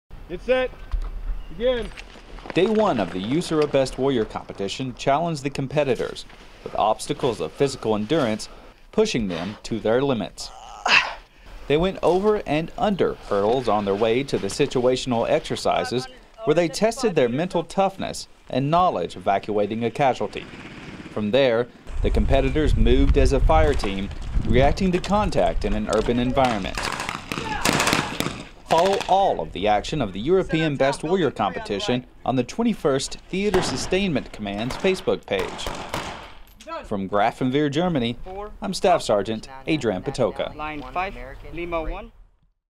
2016 Best Warrior Highlight: Day 1
The 2016 U.S. Army Europe Best Warrior candidates compete for regional supremacy.